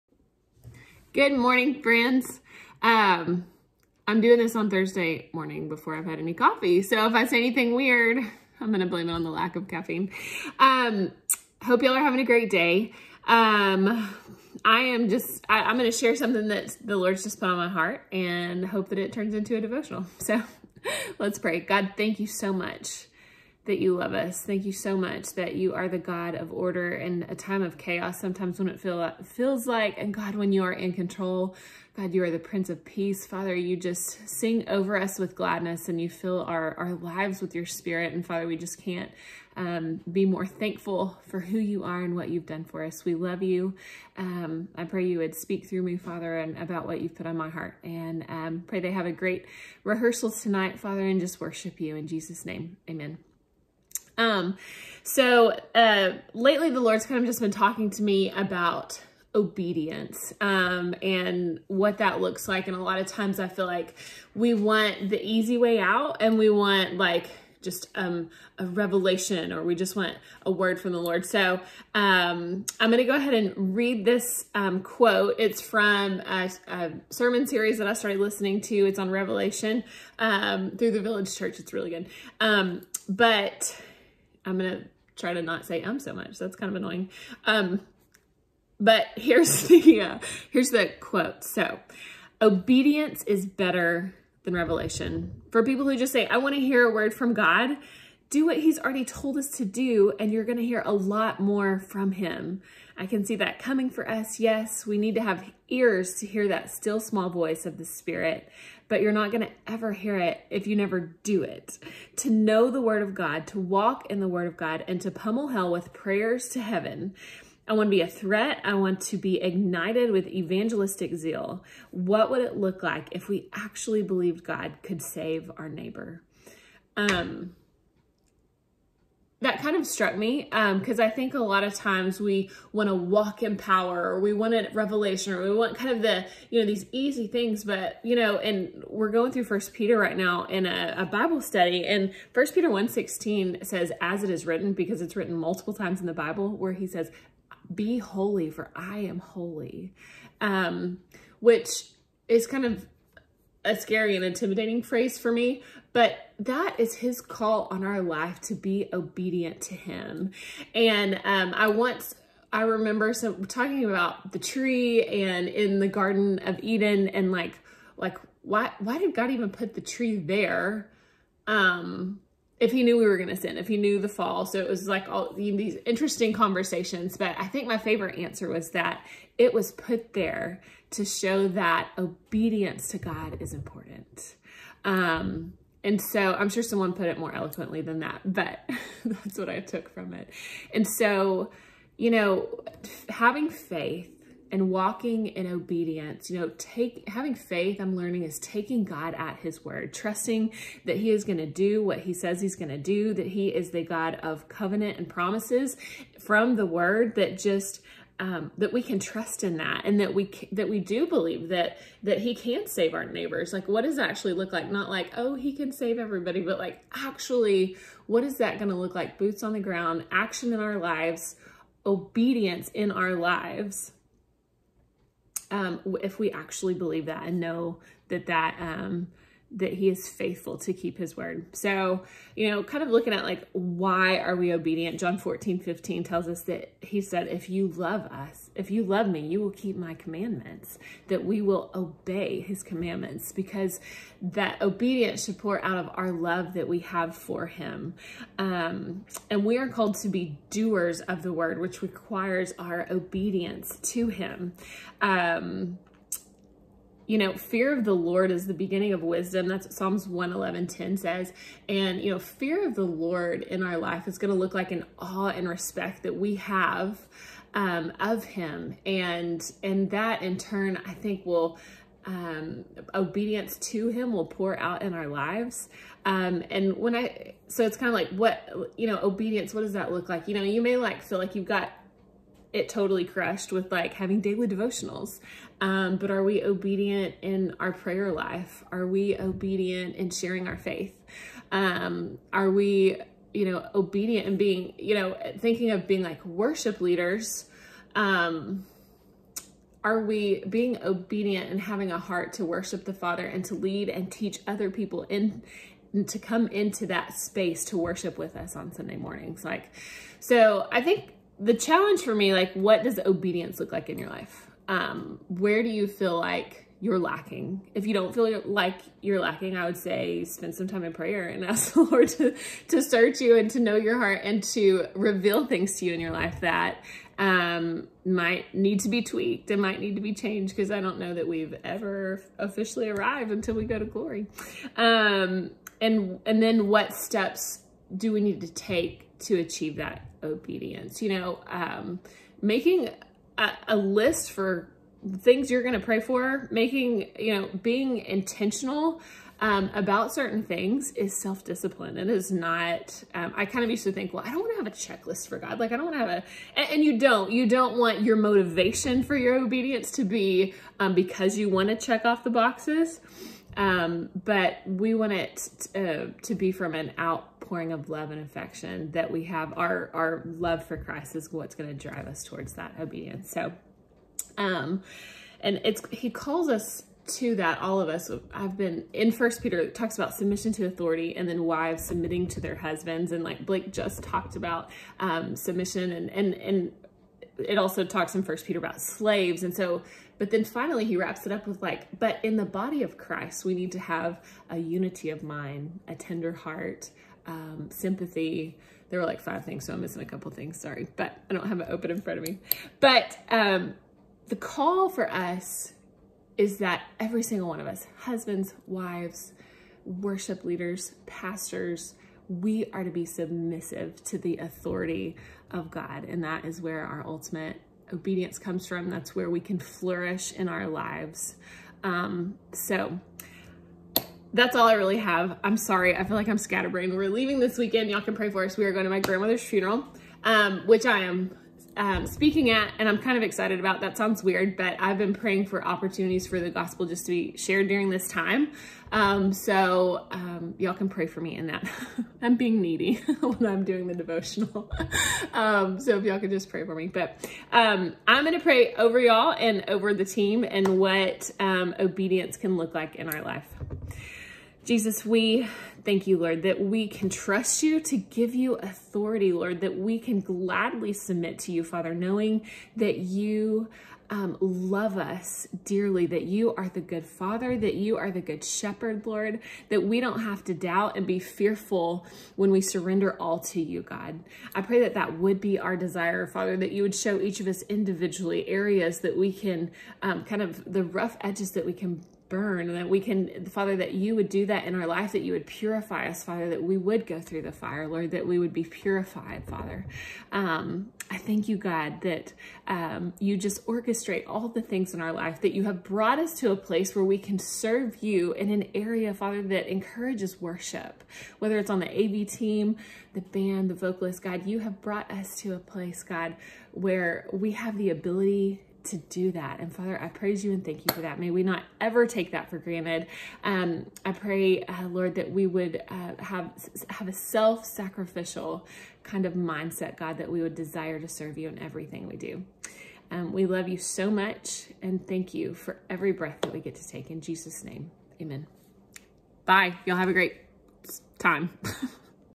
Worship Team Devotional - March 4, 2021
Tracy Cepak talks about obedience in this week's team devotional.